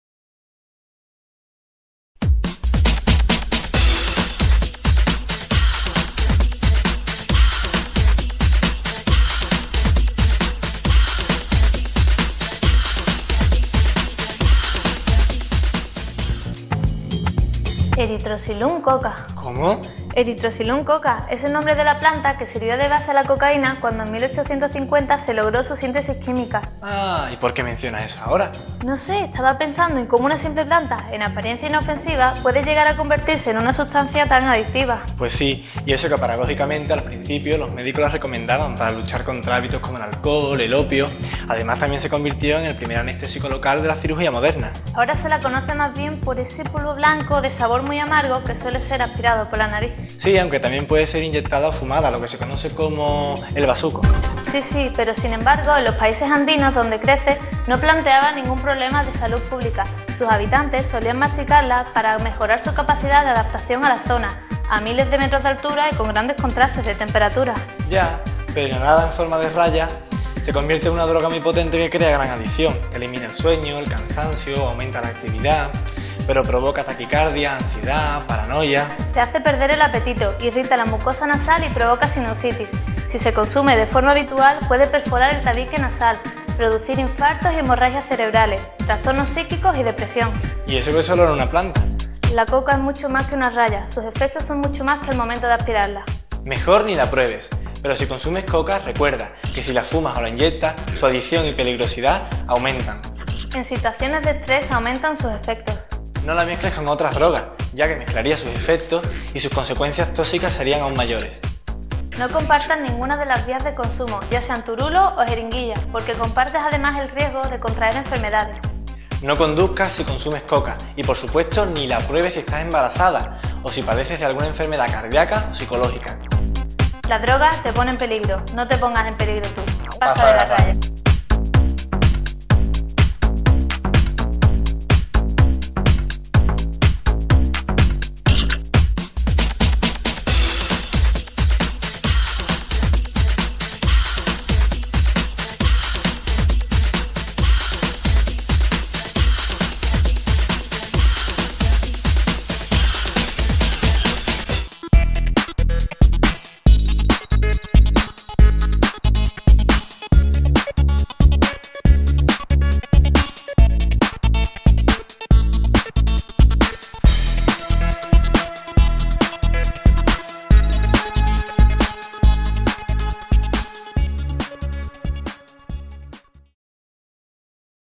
Jóvenes y consumo de cocaína (Información y prevención)
Dos jóvenes charlan sobre qué es la cocaína, efectos que produce su consumo, su poder de adicción y los daños que ocasiona. En un lenguaje cercano y directo los más jóvenes pueden informarse sobre las consecuencias que esta droga tiene sobre su salud. Este video informativo forma parte de una de las campañas de información llevadas a cabo por la Junta de Andalucía para prevenir el consumo de drogas entre los más jóvenes.